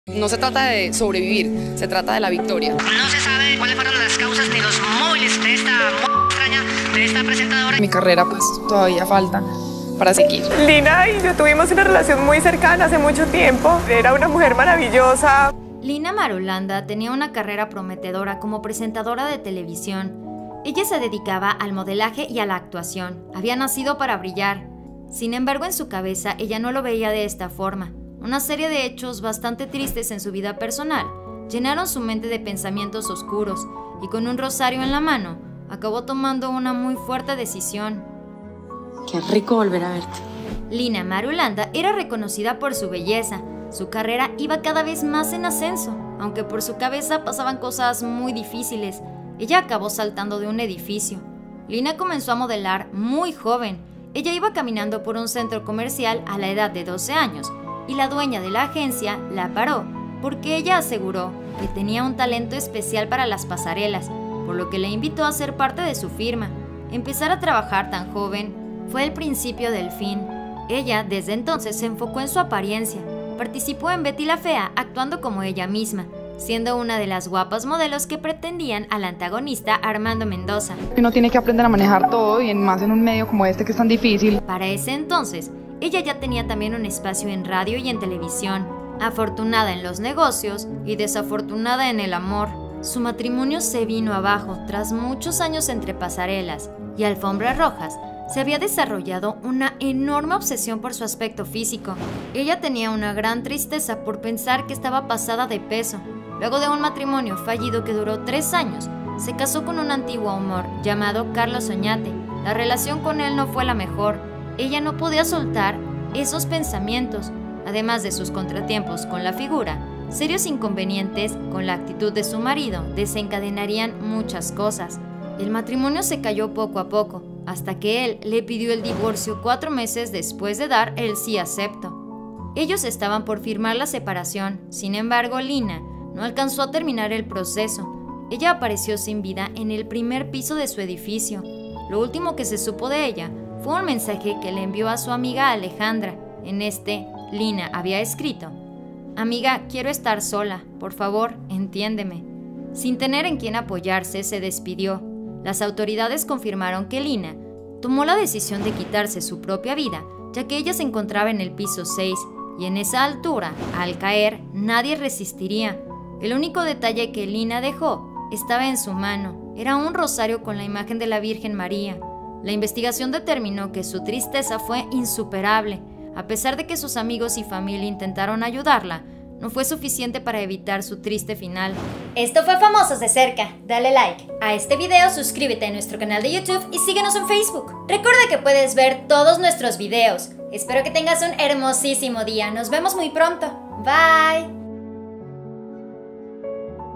🎚️🖤 𝗦𝗘 𝗤𝗨𝗜-𝗧4 𝗟𝗔 𝗩1𝗗4... !Joven Actriz de Telenovelas / 𝗨𝗟𝗧𝗜𝗠𝗔 𝗛𝗢𝗥𝗔 🖤🎚️
Se Fue Muy Joven Lina Marulanda tomo la peor decision. Aqui Todos Los Detalles.

 BIENVENIDOS!!!
suelta la sopa
televisa
exclusivas
venga la alegria
tv azteca
polemica
chismes
Entertainment
farandula mexico